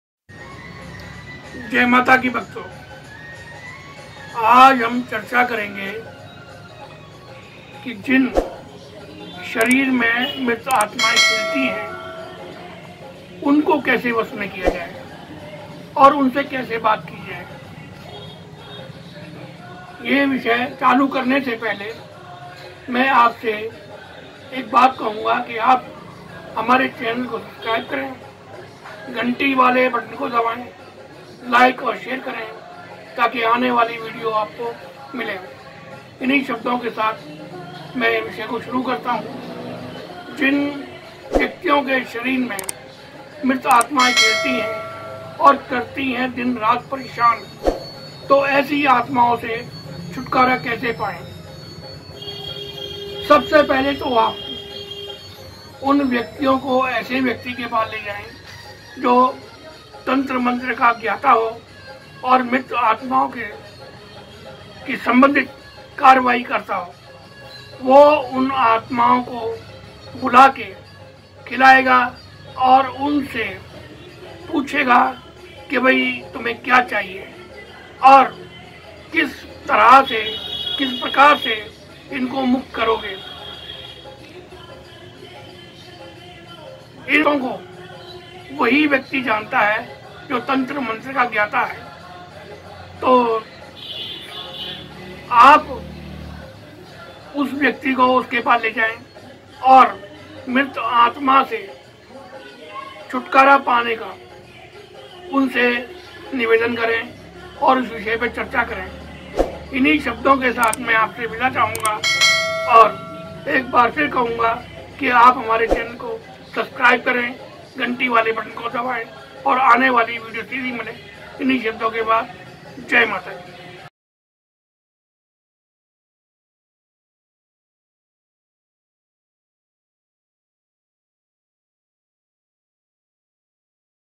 MARAT ATAMAO SE CHUTKARA PANE KE LIYE || Marat Atamao se chutkara kese paye ||Atama duriya kese paye
Q Marat atamao se chutkara kese paye
And - marat atamao se chutkara kese paye

Khelti hui atamao se baat kare or apne devtao ke madhem  se unpe dabao  banyard or apne vashibhut unko itare or samsan ya yamuna
Kinaye chhod  aye is parker se paye chutakara